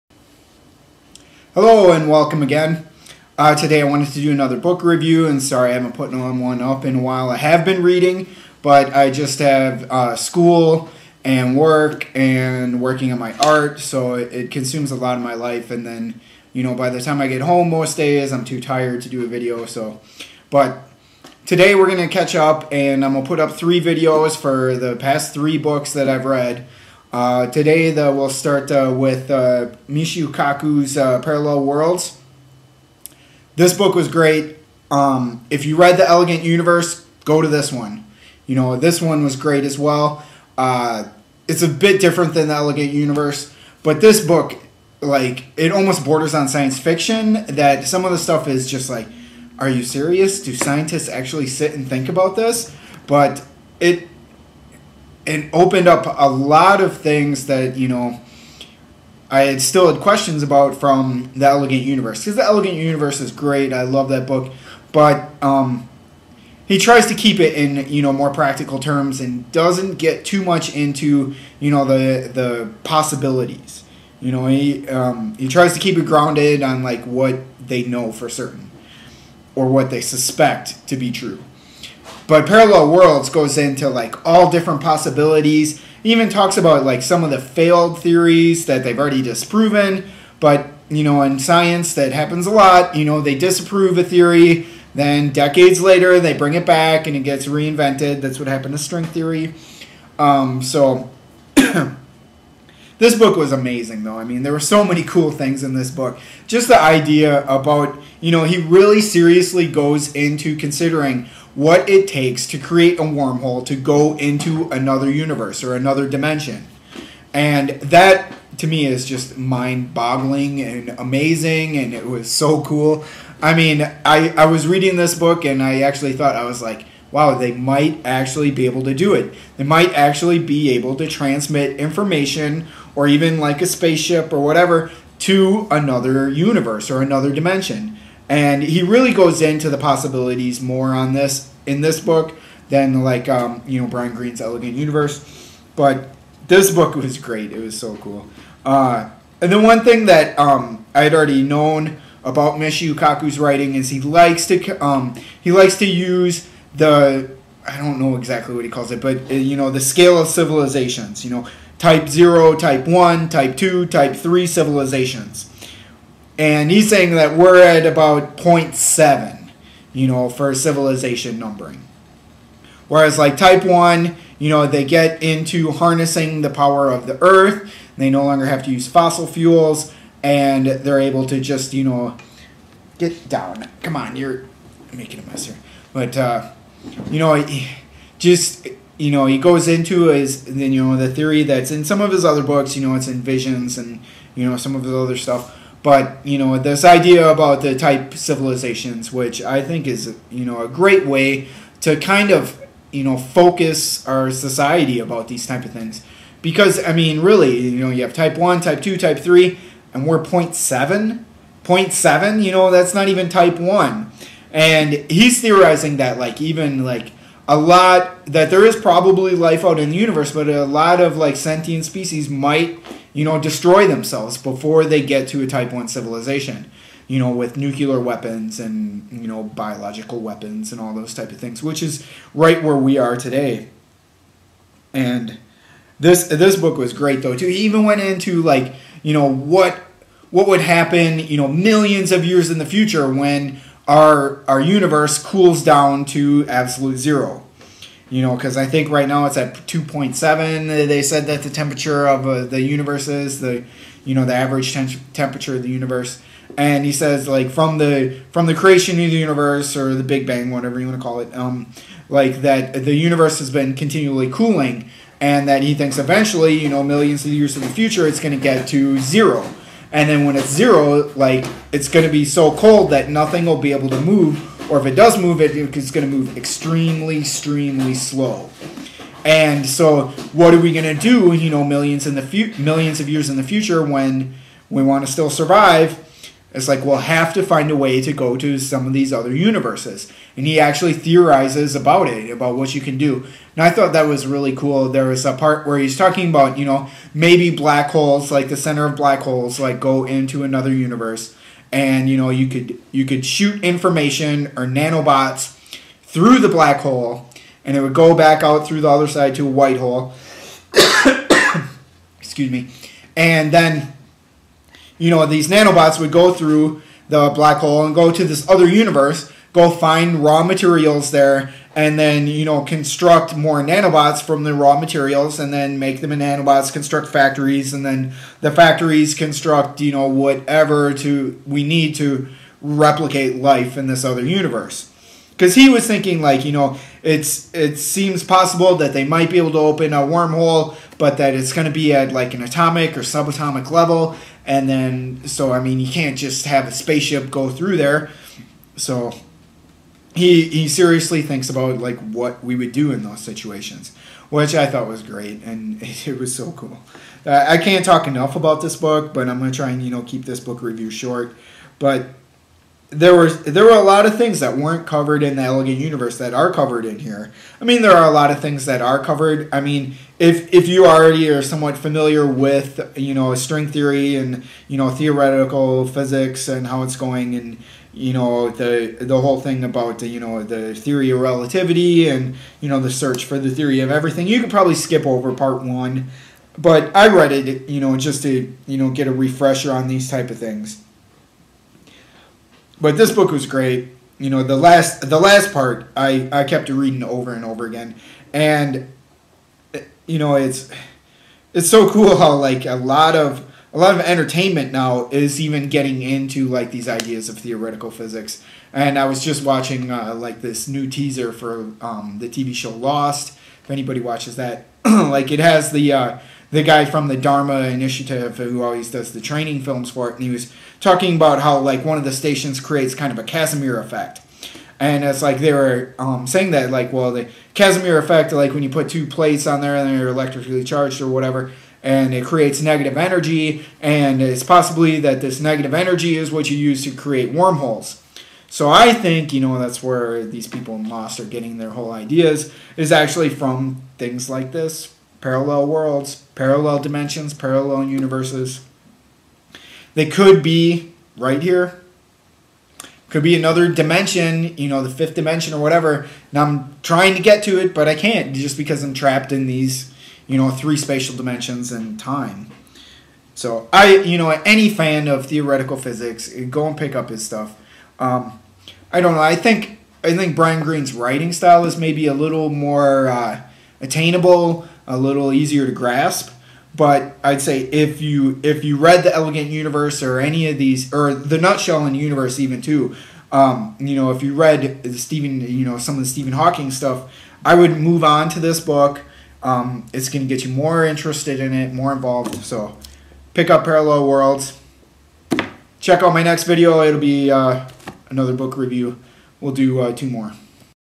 Parallel Worlds - Book Review
This is a book review of Parallel Worlds by Michio Kaku.  This is about theoretical physics and is mind blowing.

oops, at the end I meant to say the Universe in a Nutshell by Stephen Hawking.  Ha.